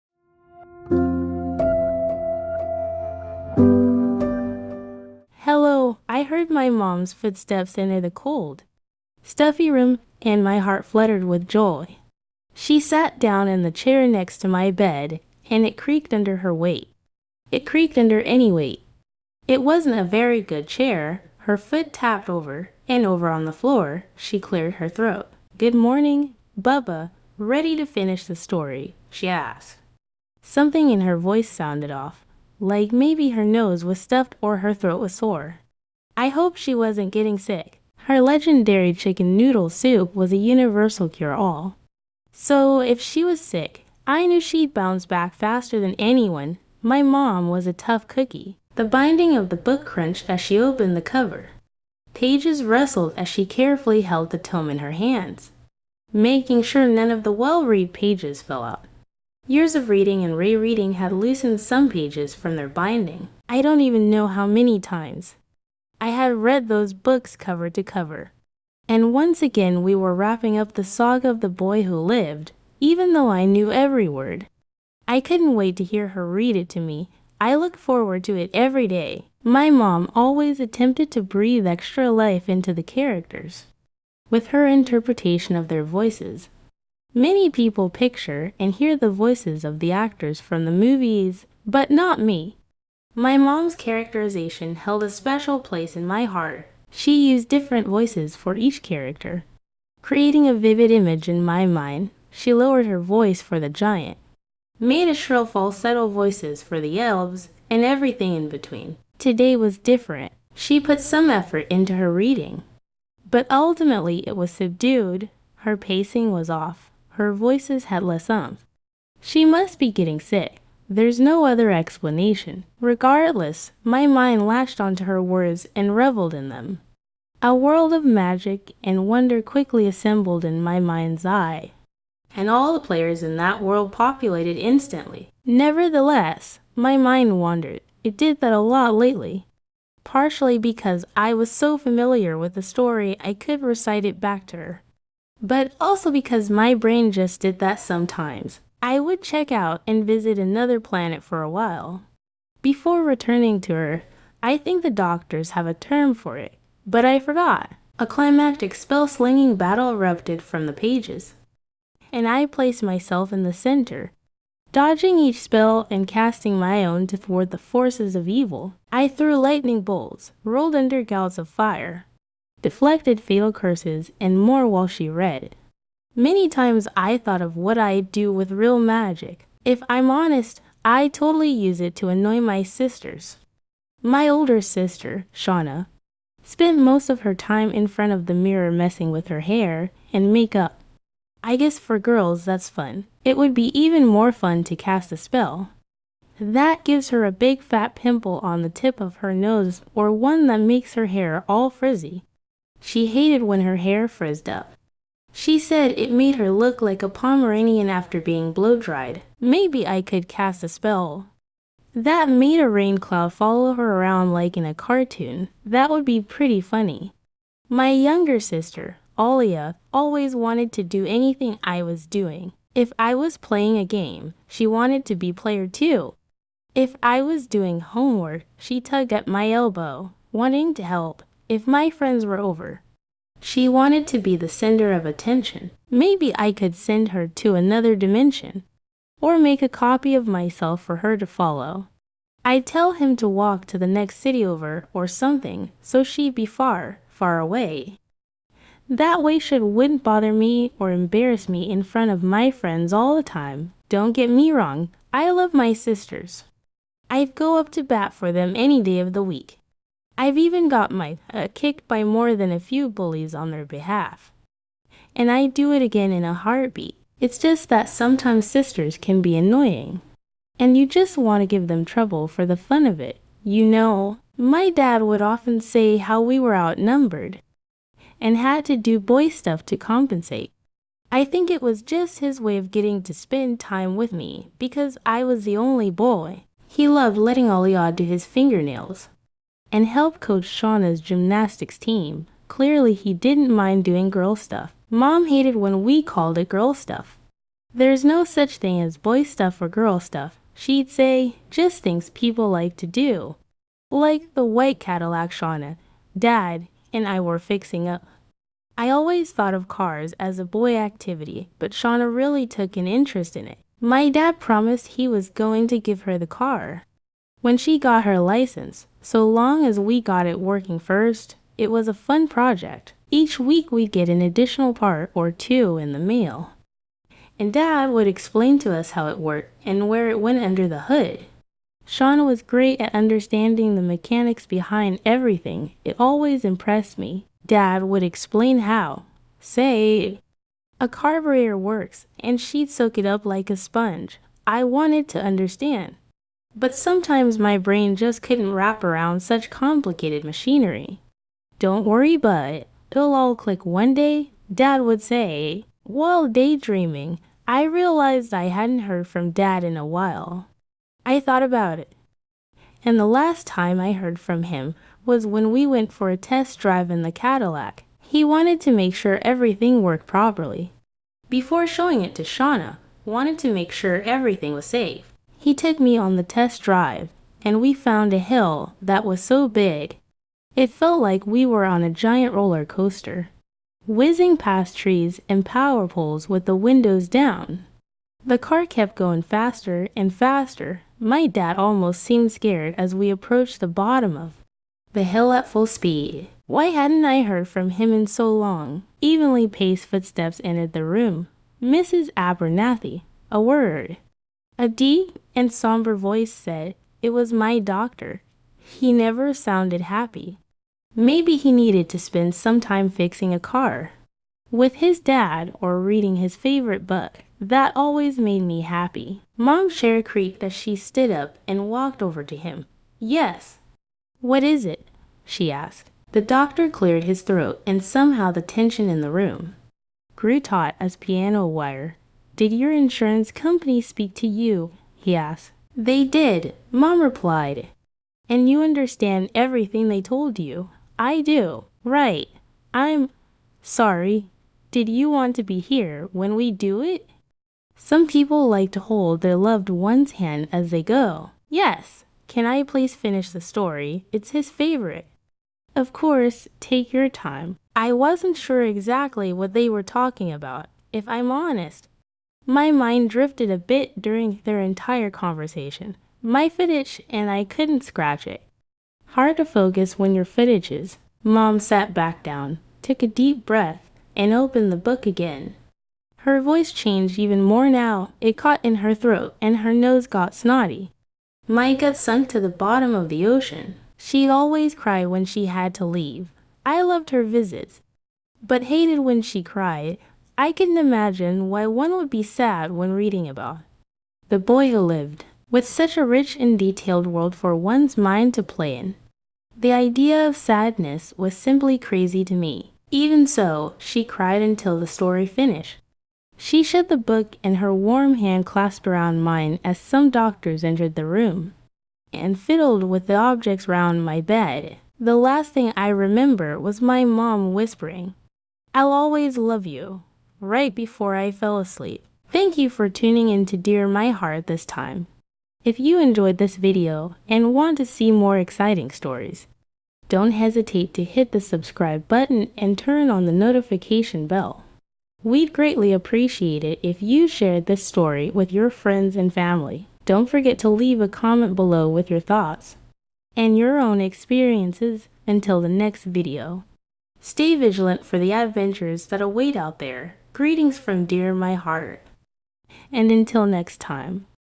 Sleeping Awake | A short story by Kyle Winter - Reedsy Stories
Sleeping Awake

The last thing I remember was my mom whispering, “I’ll always love you,” right before I fell asleep.